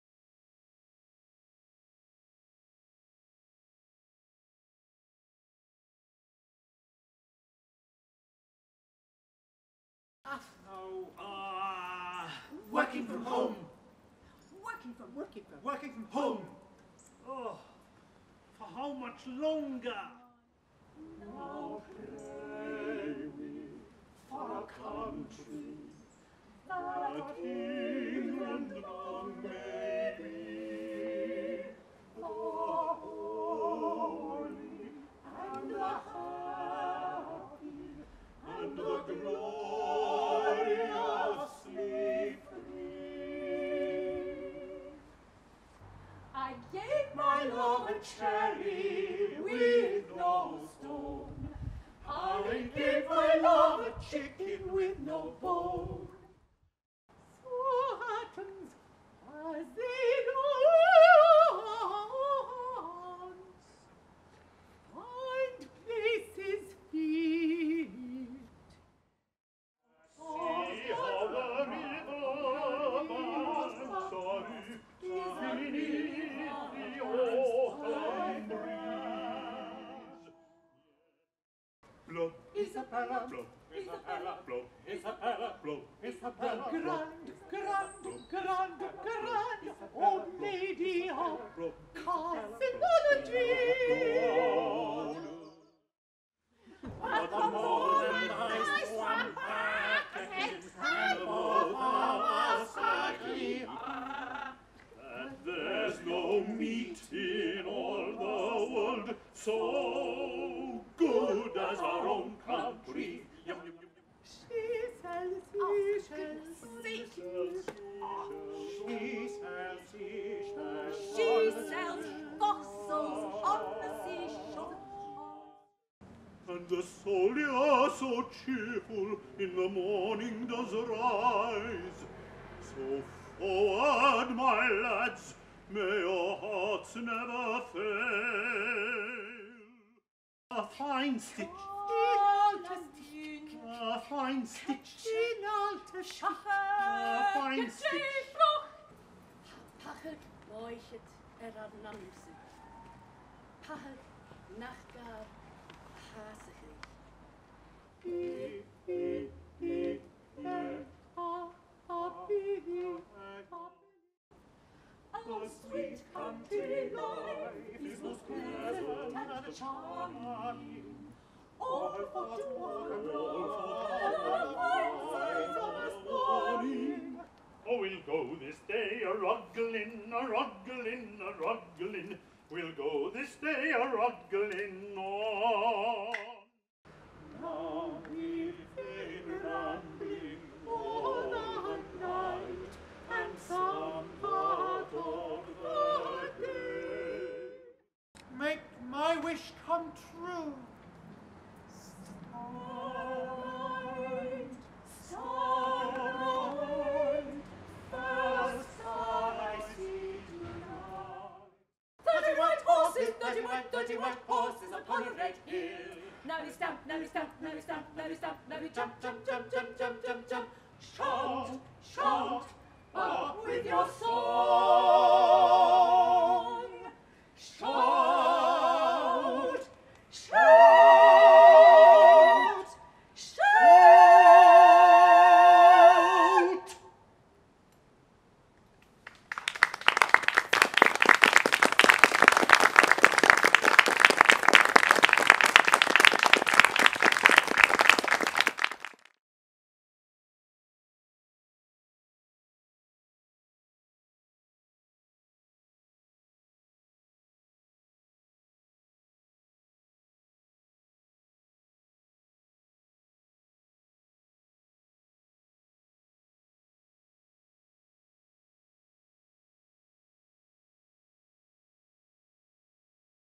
"Working From Home" - Stroud Green Festival 2021
This is a short film of extracts of the music from the only live, in-person, performance undertaken by electric voice theatre during the continuing pandemic in 2021.

“Working from Home” was a semi-staged BSL Interpreted concert, performed by an electric voice theatre acapella quartet at Stroud Green Festival on Friday September 3rd 2021. The event was staged outdoors at Holy Trinity Church, Stroud Green, London.

The music included delightful folk song settings by IMOGEN HOLST; tall tales of foreign travel by MARY MAXWELL CAMPBELL (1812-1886); stirring anthems from ETHEL SMYTH (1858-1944) and ELIZA FLOWER (1803–1846); the poetry of C16th natural philosopher MARGARET CAVENDISH and C21st Scottish Gaelic Bard, SANDY NICDHÒMHNAILL JONES; Children’s Rounds by NICOLA LEFANU; a curious carcinological limerick, and a potpourri of musical fun, frolics and heartache by local composer FRANCES M LYNCH.

Featured scientists include: Florence Nightingale, statistician and nurse; Gulielma Lister, mycologist; Dr Isabella Gordon, marine biologist; Mary Anning, palaeontologist; Margaret Cavendish, natural philosopher

Working from Home is part of the Voices for the Future Project by Frances M Lynch, supported by the Marchus Trust and using public funding by the National Lottery through Arts Council England.